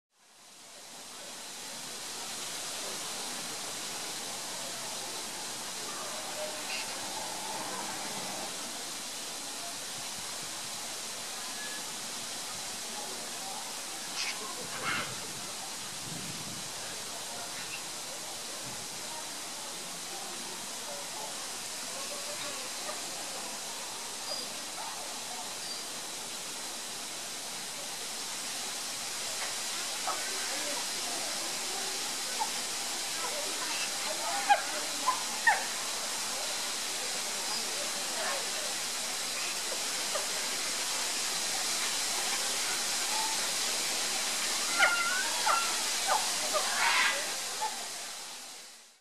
Chimpanzee lunch time at Odense Zoo
Lunch time! The chimps know the drill: run for it and collect as much as possible. And remember: No sharing!